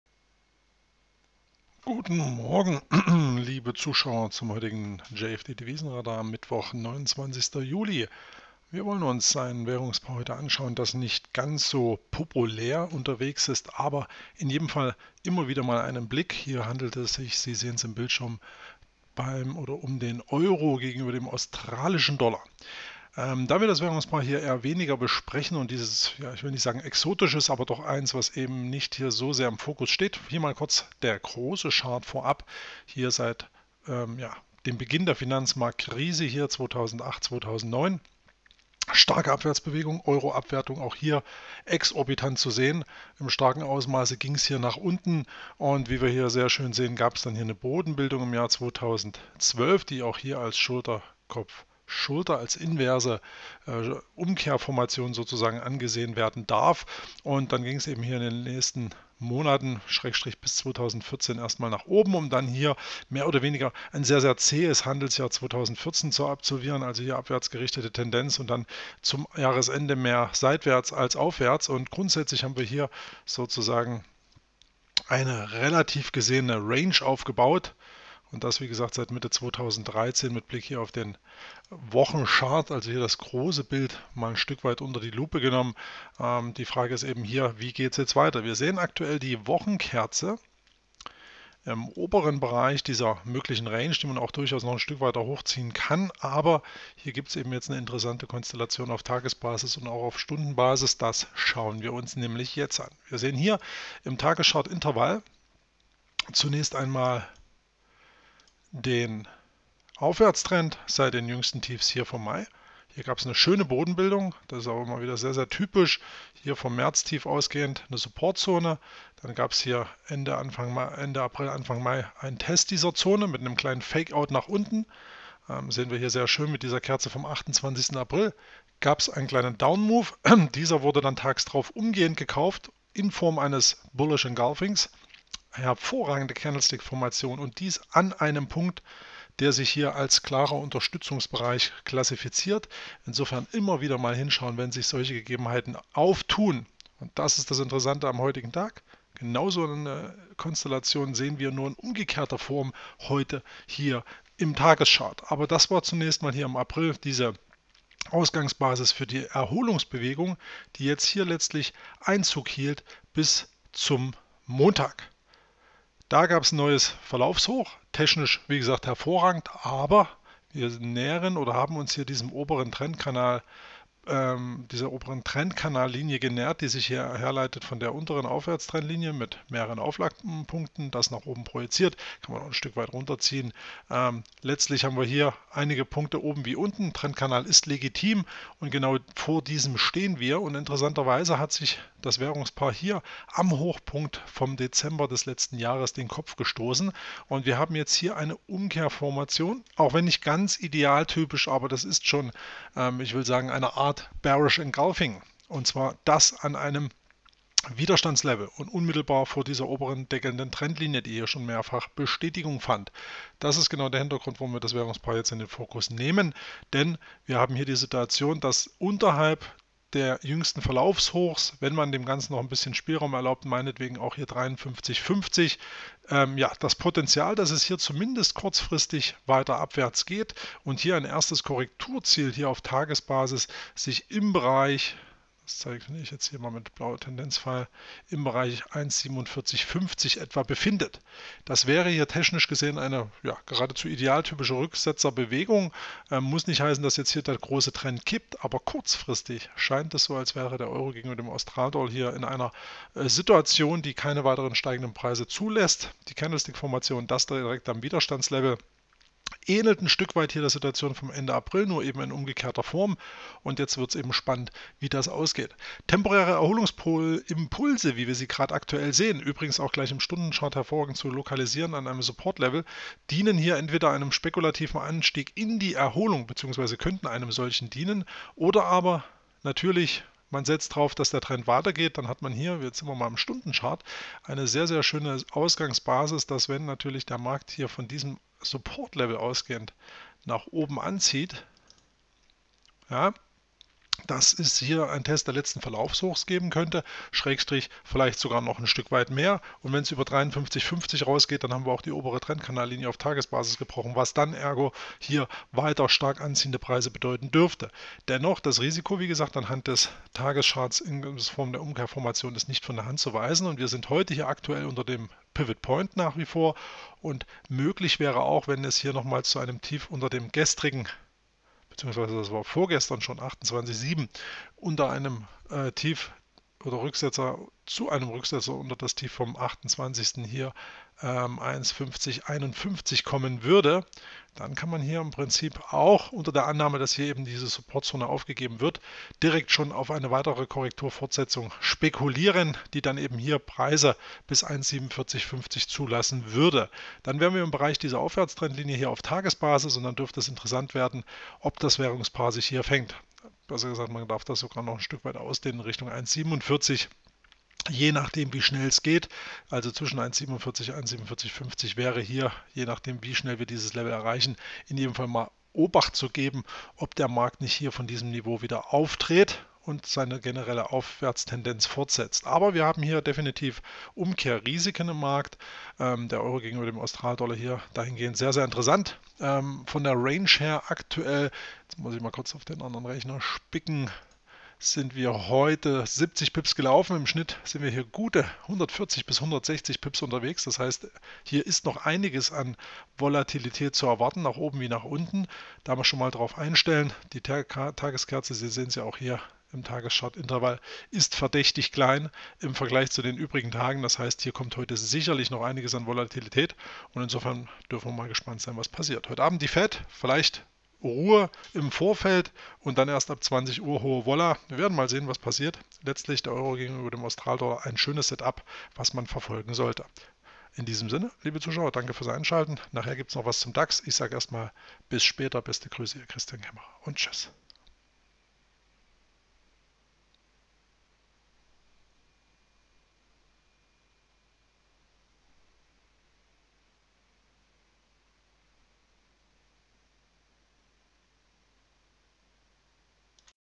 JFD Devisenradar: EUR/AUD mit bärischer Konstellation
Ist die Erholungsbewegung beim EUR/AUD bereits gereift und nunmehr ein größerer Abwärtsimpuls denkbar? Seit gestern gibt es interessante Auffälligkeiten.